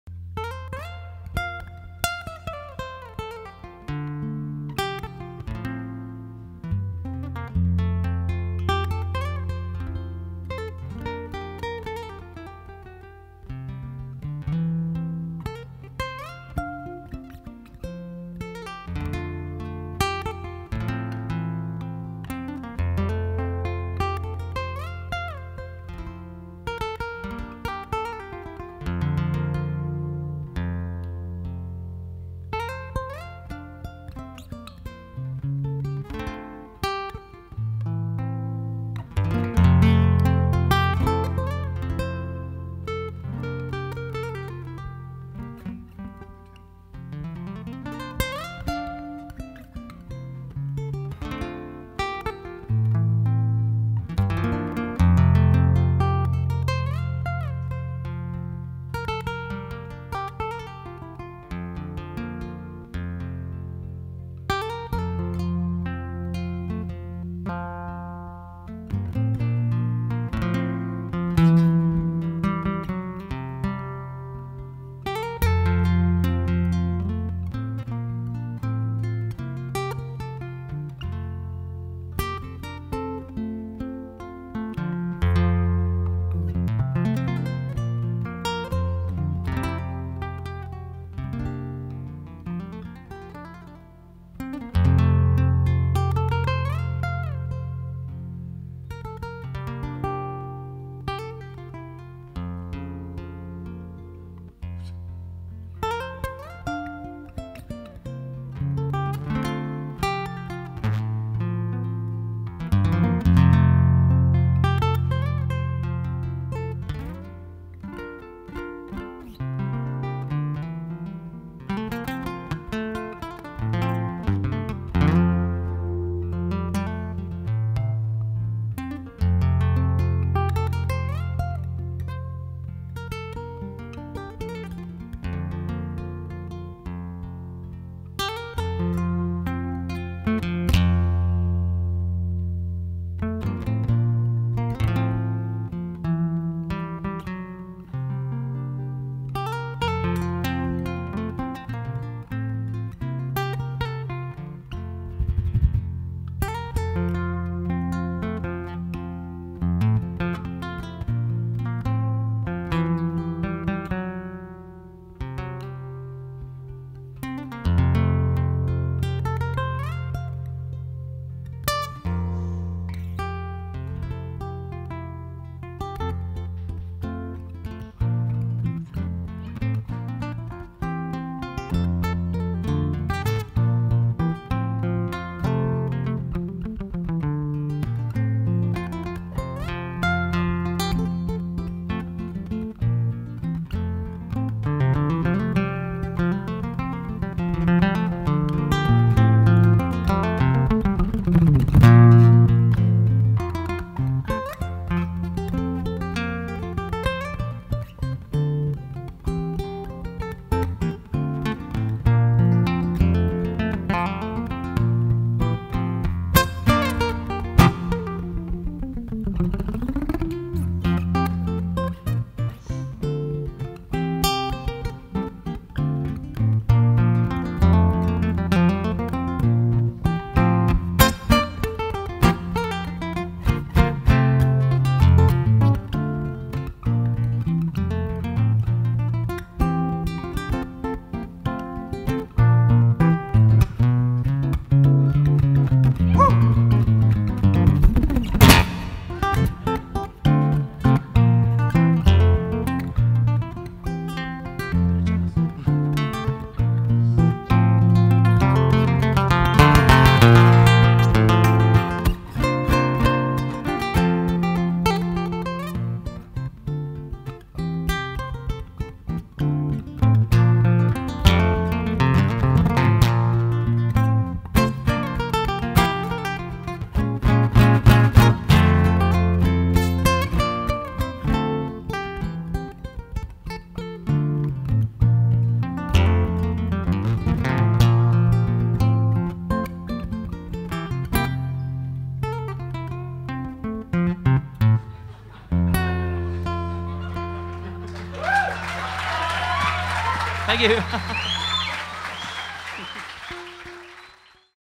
Seamus Begley & Tim Edey Clip2: Traditional Irish Music from LiveTrad.com
Get the guitar solo! The wonderful Tim Edey performs at the Frankie Kennedy Winter School at the Dunlewey Centre in Donegal in December 2010.
Connect with LiveTrad for live trad music webcasts